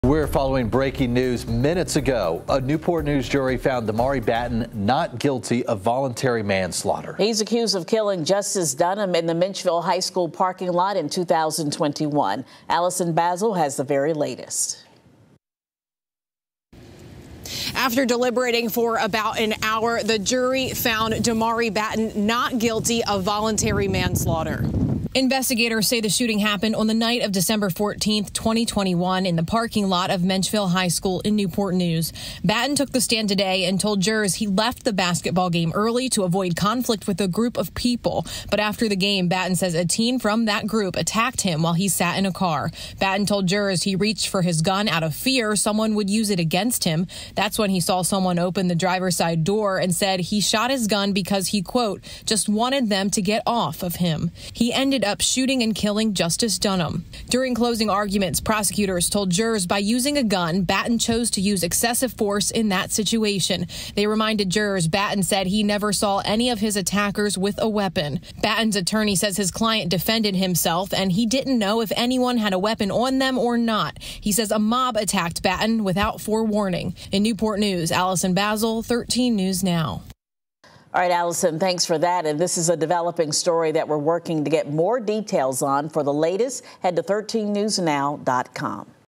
Man accused of deadly Menchville High School shooting not guilty, jury finds
A Newport News jury returned the verdict for Demari Batten one day after his re-trial began.